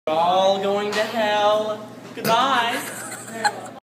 We're all going to hell meme
Everyone hates school. Ashton hate school so much that instead of calling it school he called it's hell.